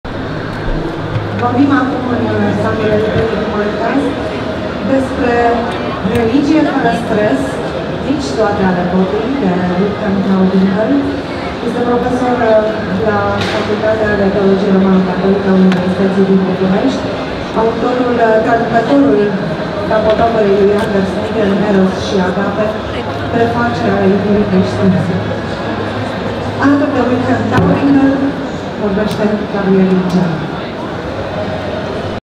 (01) Gabriela Maaz - Lansare de carte "Nici toate ale popii. Religie fără stres" de Wilhelm Tauwinkl
În data de 23 noiembrie 2019 a avut loc lansarea de carte "Nici toate ale popii. Religie fără stres" de Wilhelm Tauwinkl în cadrul târgului Gaudeamus 2019.

Vorbitori: Wilhelm Tauwinkl, Gabriel Liiceanu.